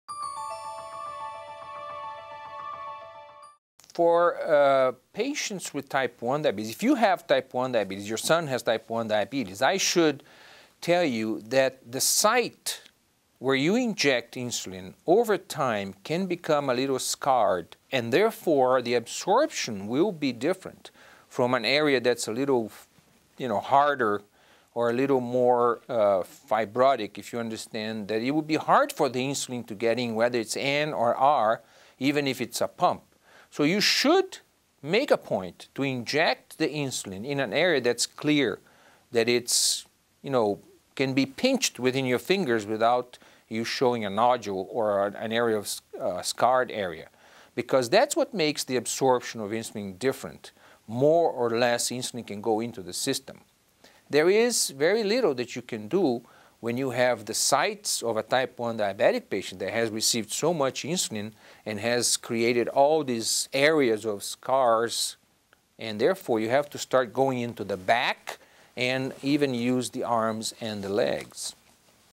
How Can We Avoid Diabetes Injection Site Scarring?
Learn about diabetes injection site scarring. MDs answer your diabetes questions.